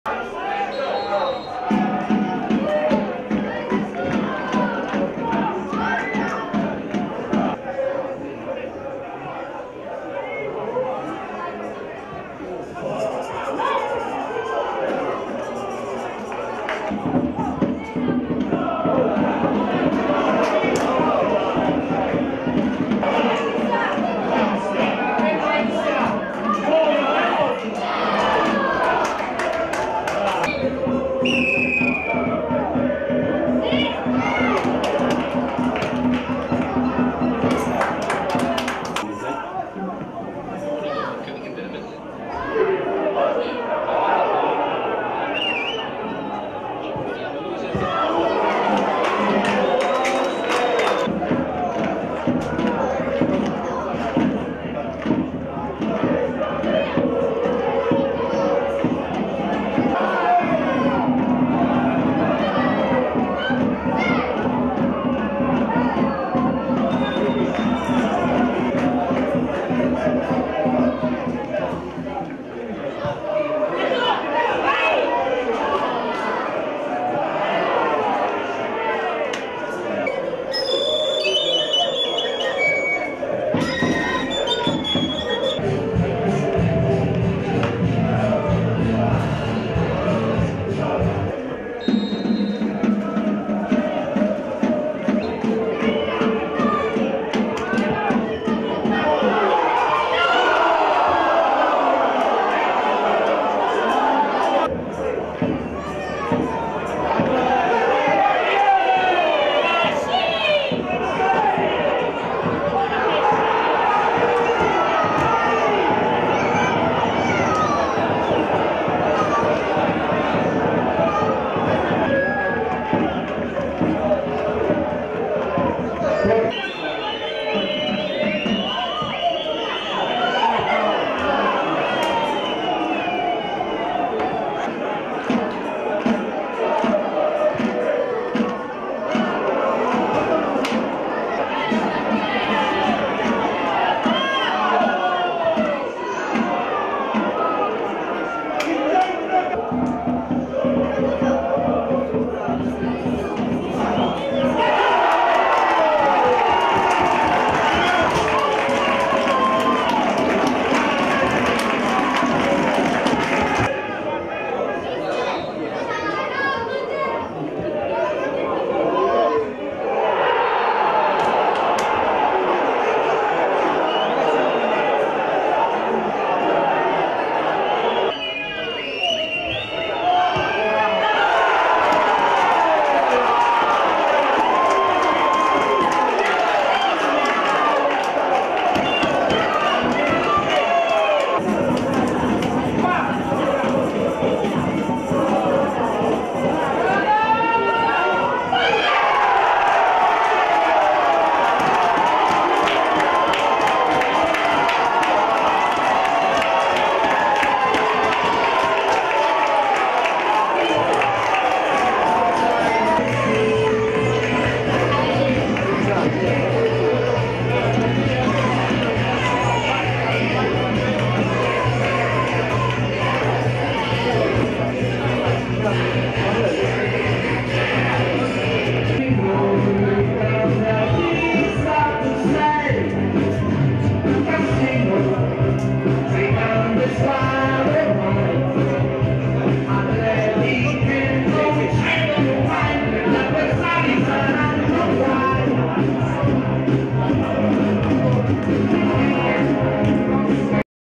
Calcio Serie D Gir. G - Cassino-Nola 3-2 (Highlights)
🏆⚽ Calcio Serie D 2022/2023
📅 30° giornata

ASD Cassino Calcio  🆚 Società Sportiva NOLA 1925

Le immagini della gara🔥